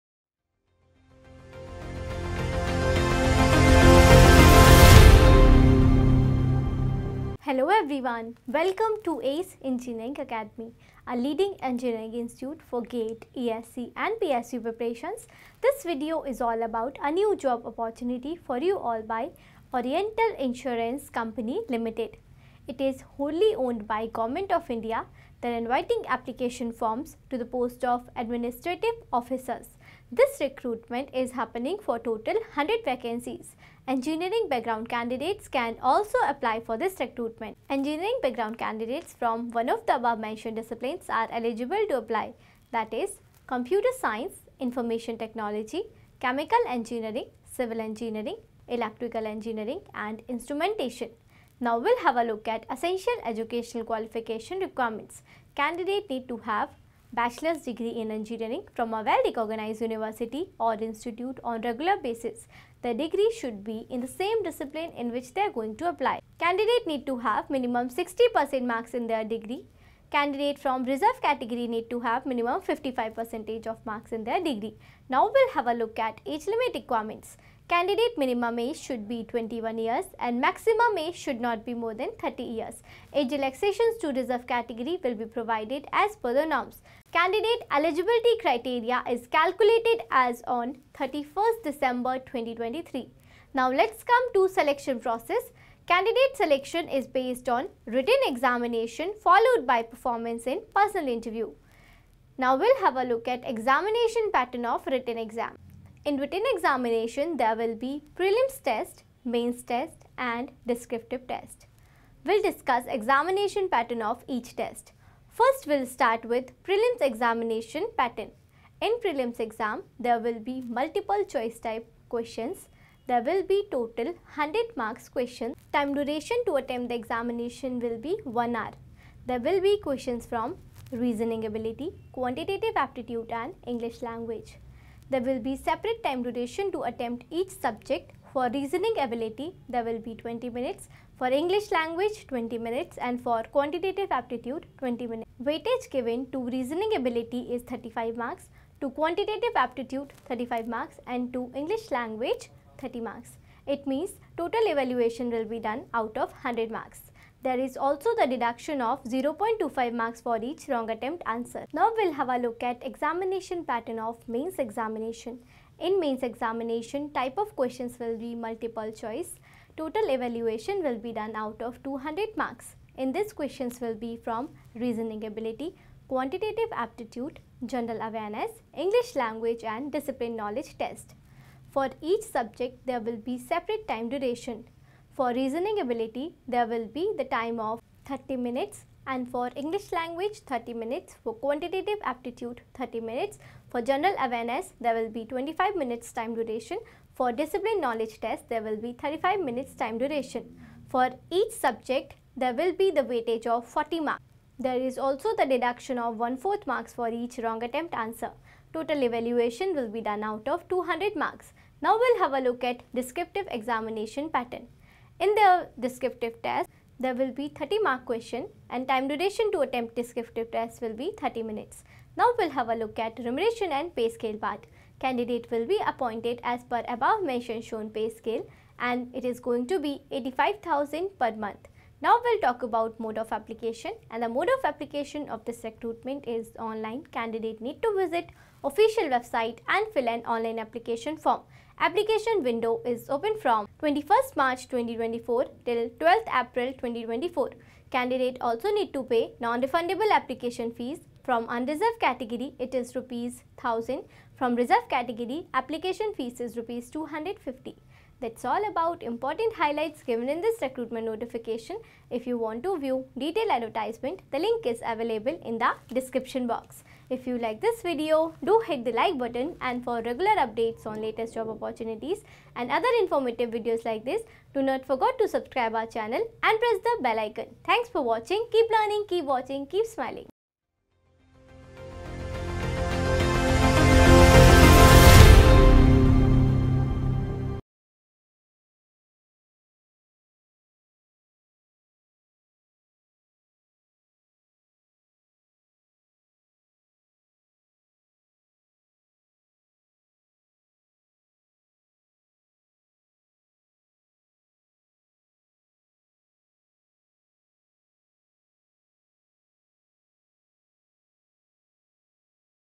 OICL invites the application forms to the post of Administrative Officer | Last Date th April
Oriental Insurance OICL invites the application forms to the post of Administrative Officer Engineering candidates can also apply

POST NAME:
Administrative Officer

DISCPLINE
 Information Technology
 Computer Science
 Chemical
 Electrical
 Mechanical
 Instrumentation
 Civil

EDUCATIONAL QUALIFICATION:
Full Time Degree in Engineering from a recognized university/Institute

AGE LIMIT
Age relaxations as per norms

SELECTION PROCESS
 Written Exam
 Personal Interview

EXAM PATTERN
Written Exam:

Prelims Exam:
Question Type: MCQs
Time Duration: 1 hour
Total Marks: 100
Total Question: 100
Negative Marking of 1/4th Marks
Subject: Reasoning: 35 Marks/20 Minute,
English: 30 Marks/ 20 Minute, Quantitative
Aptitude: 35 Marks/ 20 Minute

Mains Exam:
Total Marks: 200
Time Duration: 2.5 Hours
Type of Questions: MCQs
Negative Marking: 1/4th Marks
Subjects:
 Reasoning: 40 Marks/ 30 Minutes
 English: 40 Marks/ 30 Minutes
 Quantitative Aptitude: 40 Marks/ 30 Minutes
 General Awareness: 40 Marks/ 25 Minutes
 Discipline Knowledge: 40 Marks/ 35 Minutes
 Descriptive Test: 30 Marks/ 30 Minutes

Personal Interview

PAY SCALE
85000/- Rs Per Month

Mode: Online
Application Begin : 21/03/2024
Last Date for Apply Online : 12/04/2024

Application Fees:
General / OBC / EWS : 1000/-
SC / ST / PH : 250/-